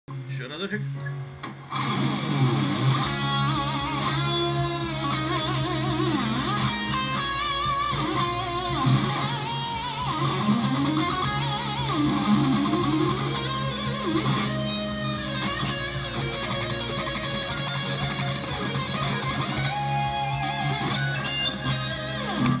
Разучиваю соло с учеником :)
Готовим соло, решили добавить кое-чего, пришлось и самому немного поиграть :)